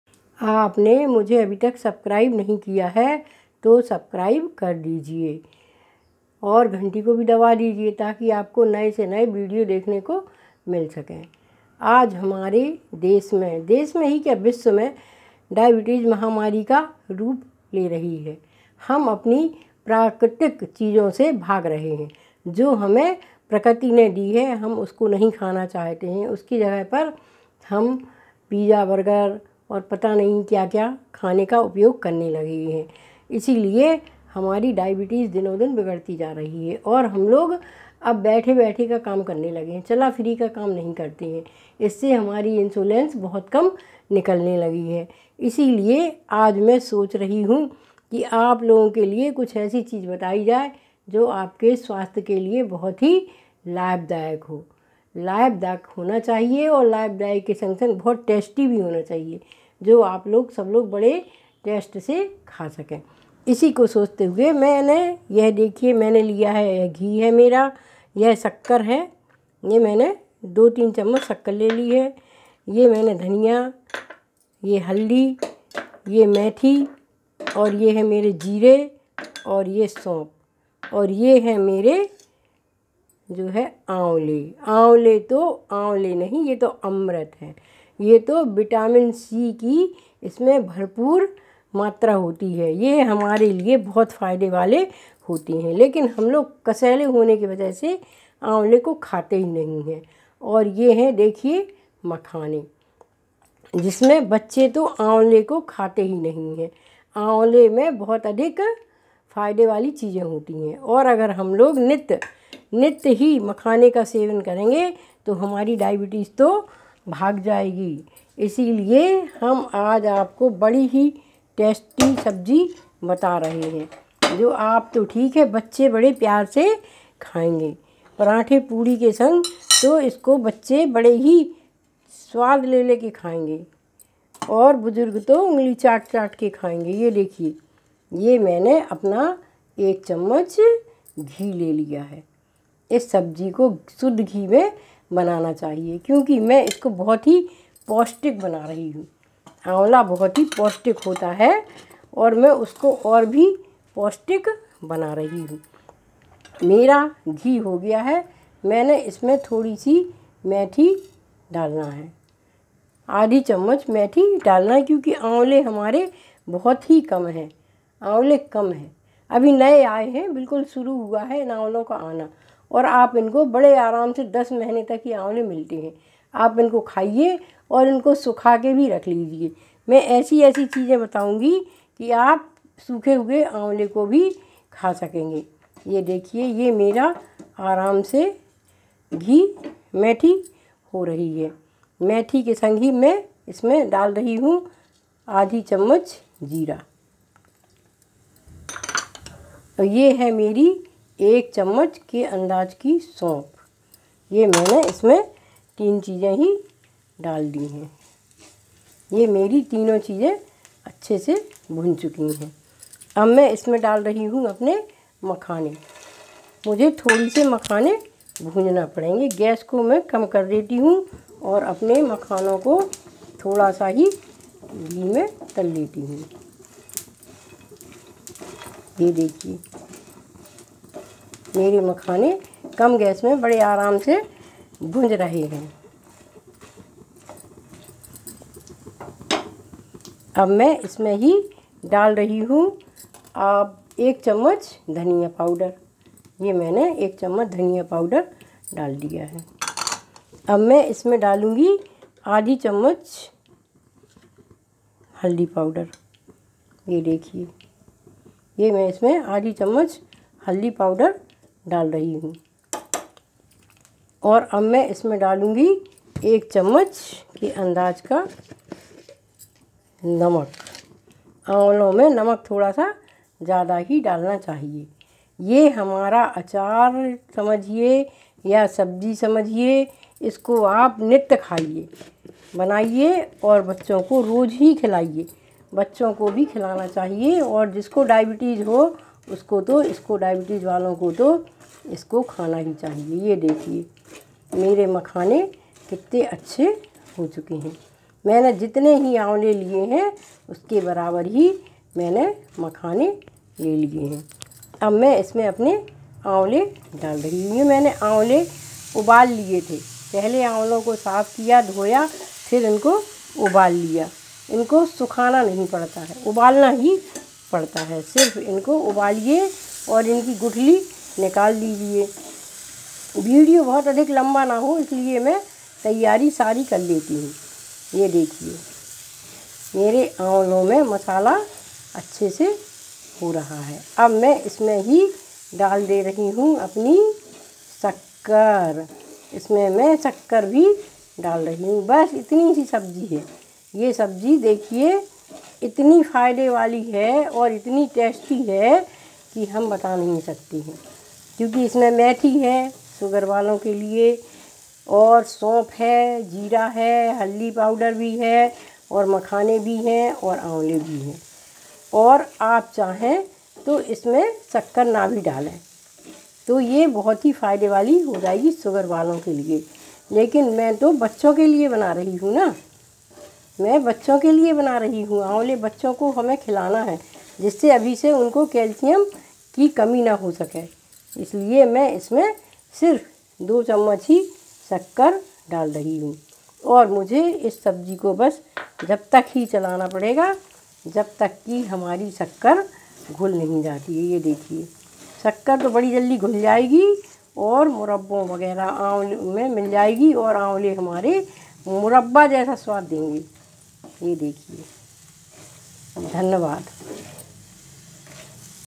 हैल्दी हैल्दी खट्टी मीठी सब्जी सरल विधी से बनाईये डायविटीज को भगाईये
मखाने एवं आंवला की खट्टी मीठी सब्जी सरल विधी से बनाईये एक बार बनायें गे तों बार बार बनायें गेजरूर